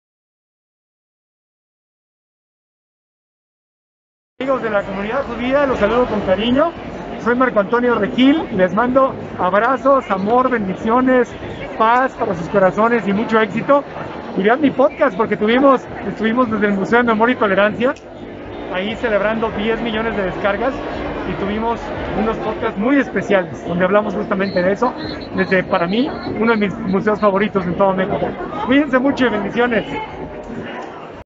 Abrazos Amor Bendiciones y mas para la comunidad Marco Antonio Regil
Como cada semana otra personalidad nos comparte su mensaje de Shabat A gut Shabes para todos